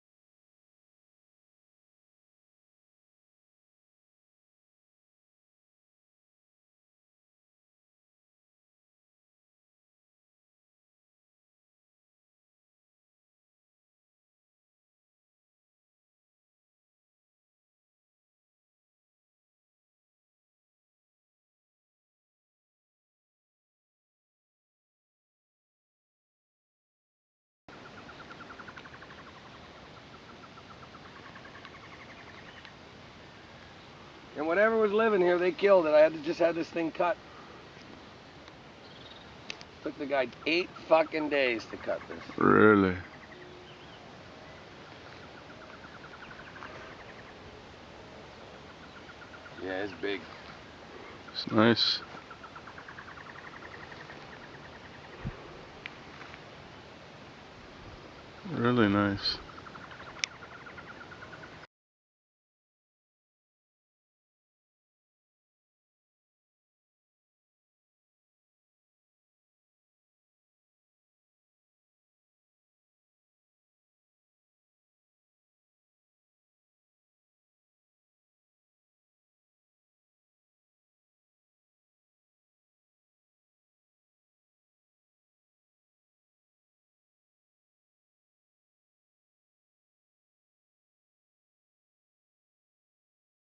Vista Ridge Golf and Country Club Lot 17 For Sale
Vista Ridge Golf and Country Club Lot 17 For Sale.  Fairway, green and Mountain view lot.